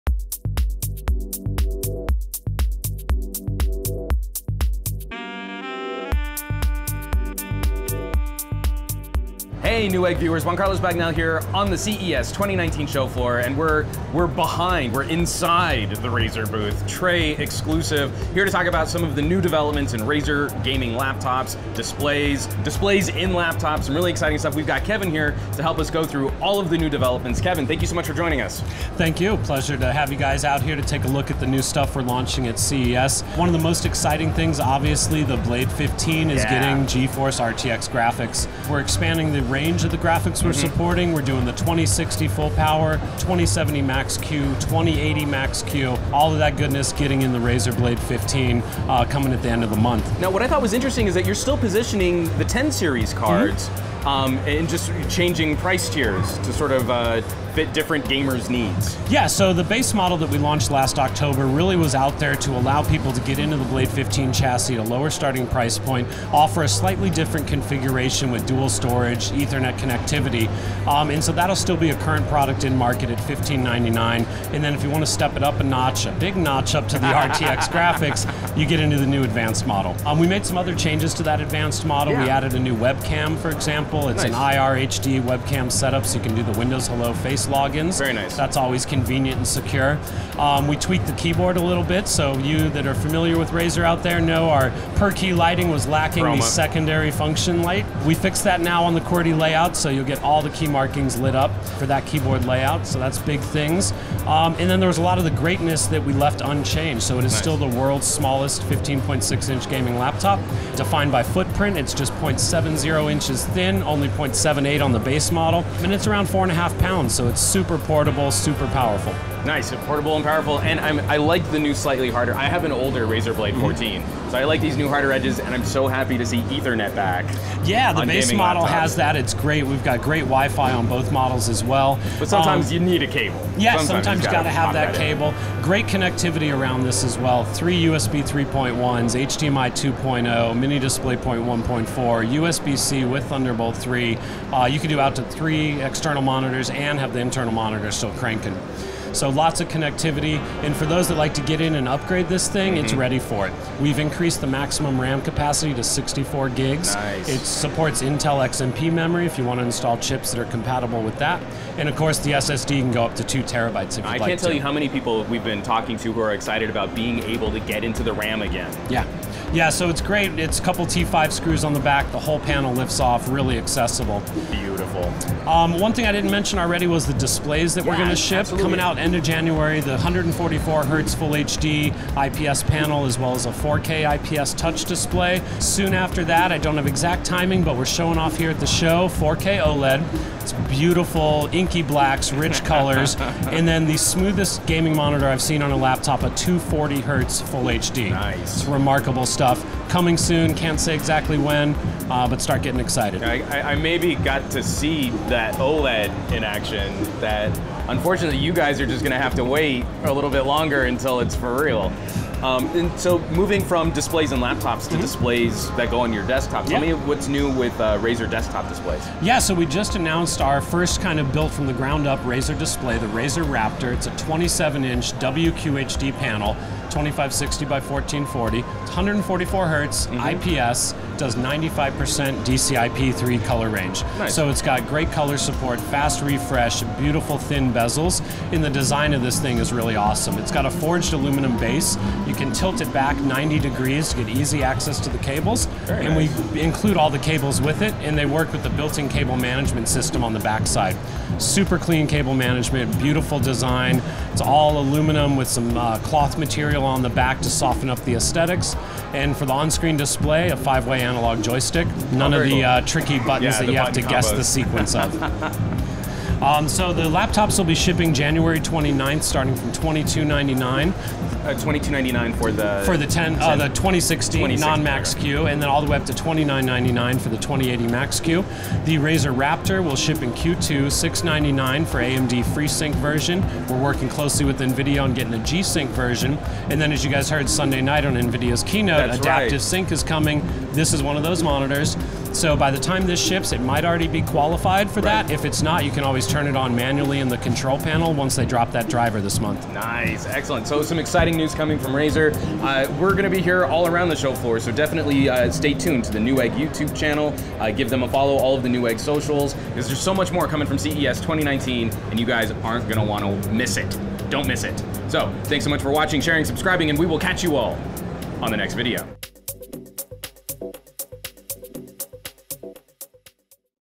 Razer Unleashes the Raptor Monitor at CES 2019
Razer’s first desktop gaming display lit up their booth at CES 2019, along with all their newest RTX powered Blade laptops. Join JC Bagnell and Kevin Sather as they dive deep into Razer’s 2019 gaming lineup.